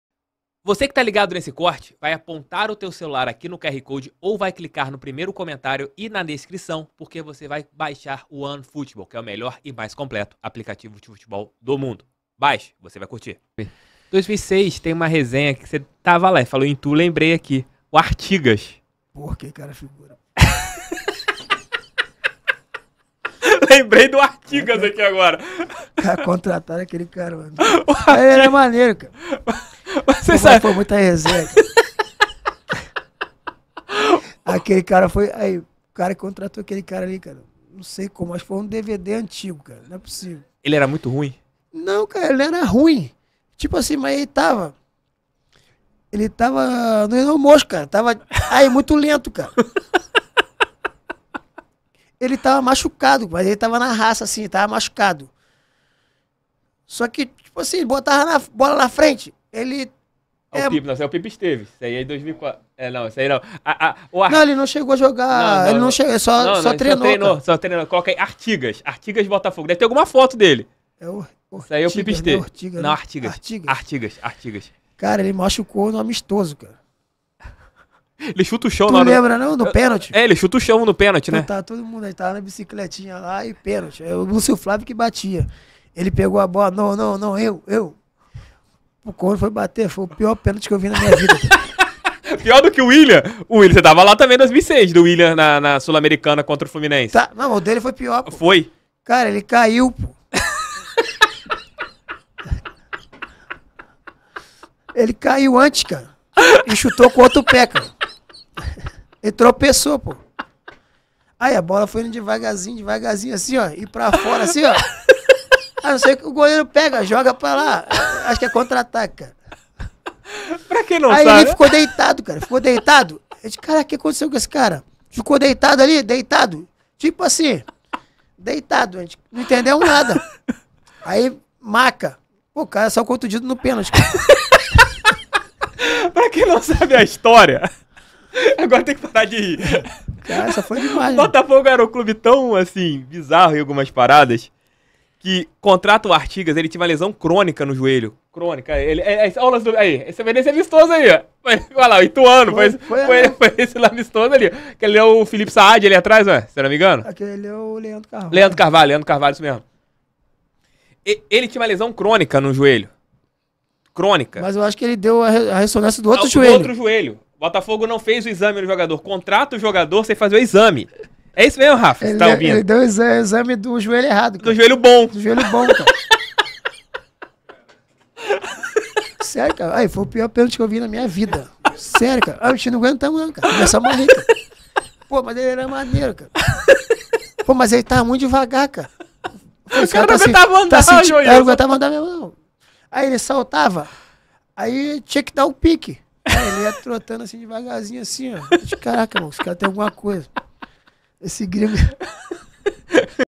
UM DOS REFORÇOS MAIS BIZARROS DA HISTÓRIA DO BOTAFOGO
Joílson fala sobre a passagem do uruguaio Jorge Artigas pelo Botafogo em 2006 e relembra momento grotesco protagonizado pelo jogador num amistoso contra o Ituano